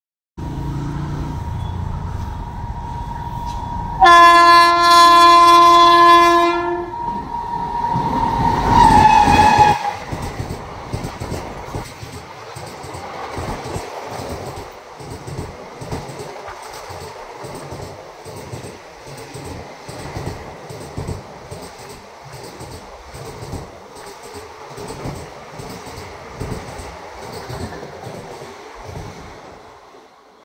SMVT Bangalore-Danapur Special train, powered by WAG 9 skips Hoodi at an astonishing speed
Camera Used
Cannon SX70

Rail Travelers